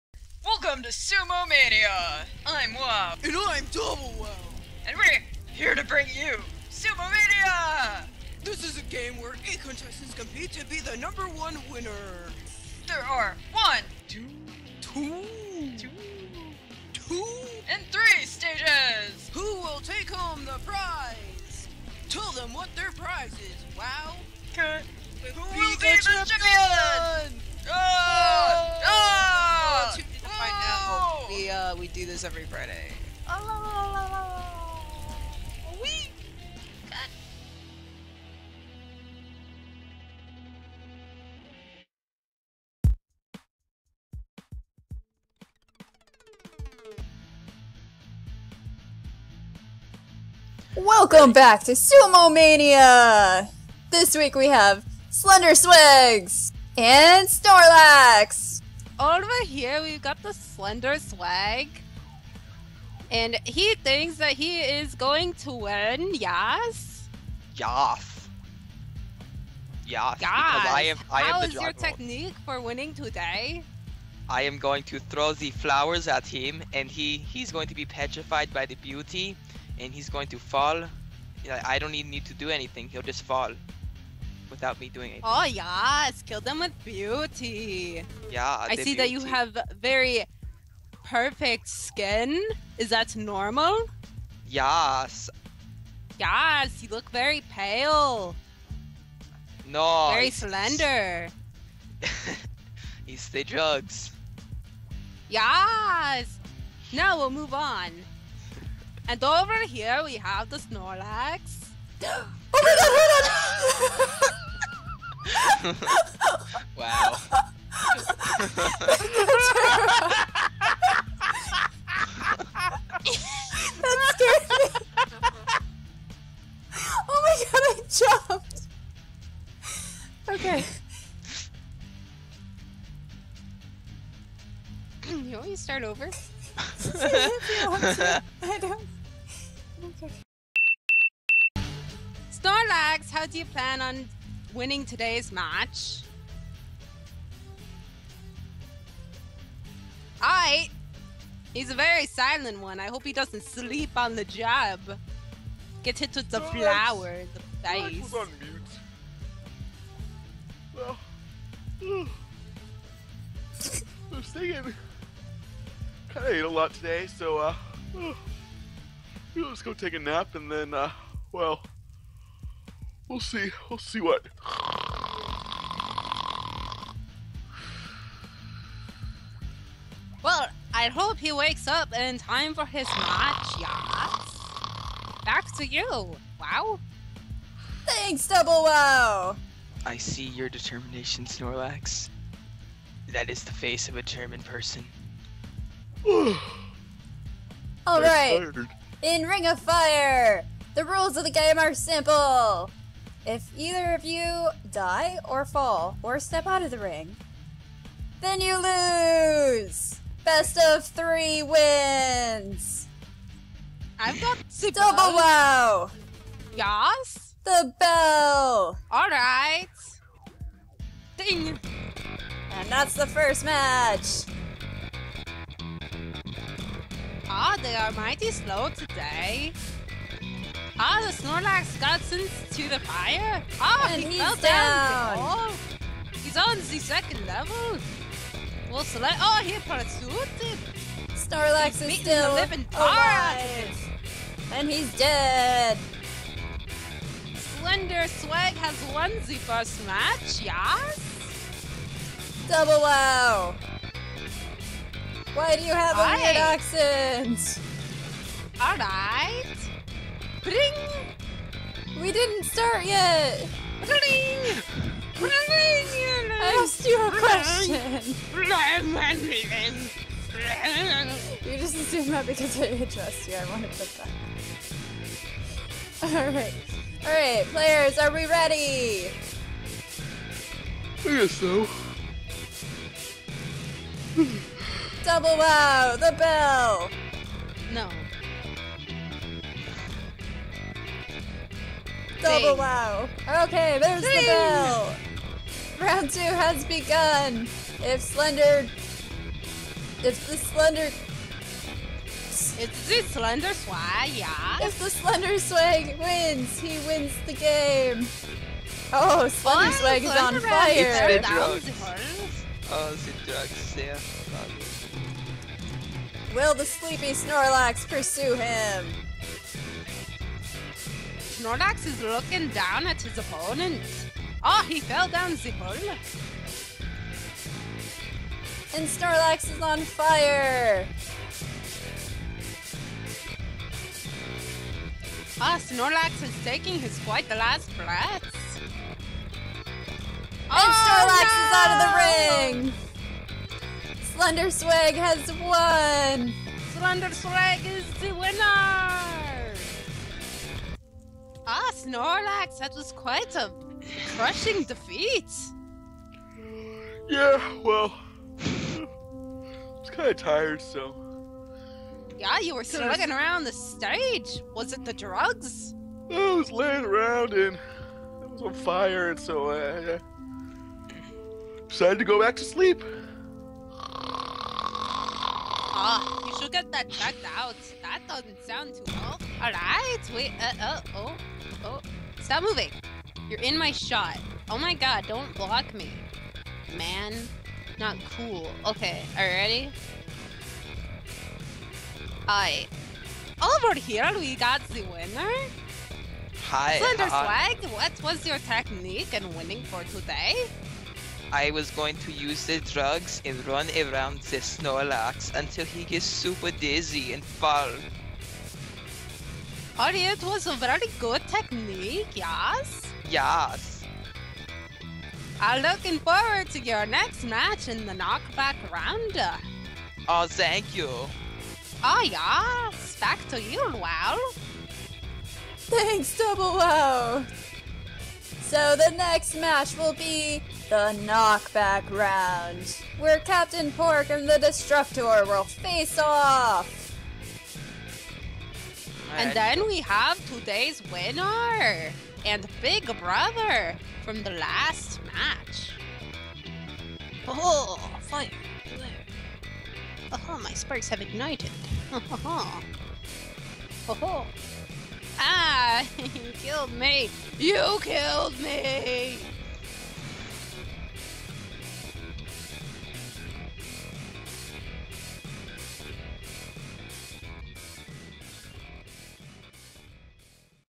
SUMOMANIA - S1E4 - Snorlax vs SlenderSwegz
DO YOU LIKE SUMO??

DO YOU LIKE ROCK??

If your answer is "yes" to both these questions, then BUCKLE UP FOR SUMOMANIA!!

SUMOMANIA is a game show that features the popular game "sumo" in a tournament style competition!  There will only be one champion of SUMOMANIA!  Who will win this season's competition?